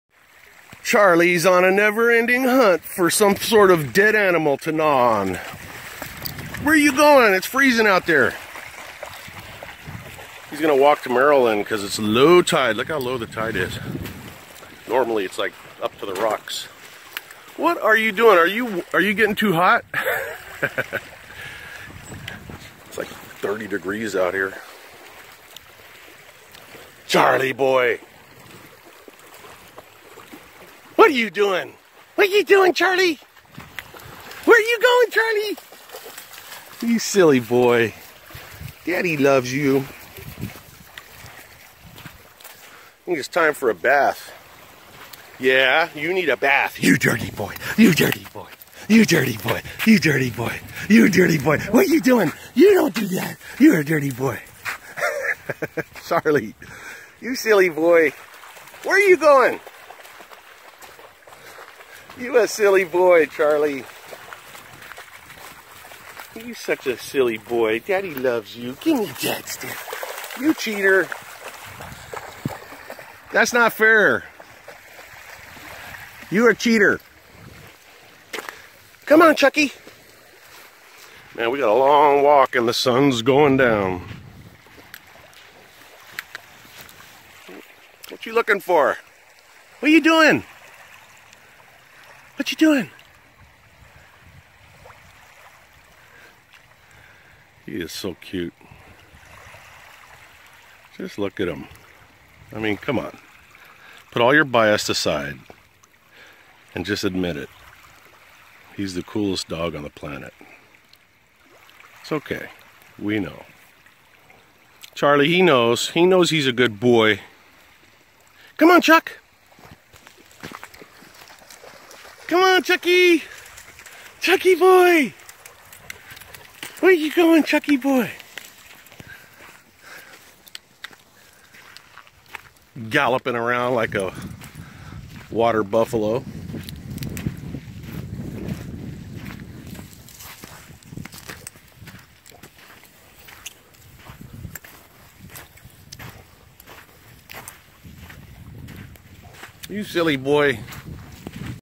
Hanging With My boy
Charlie and I on our daily walk.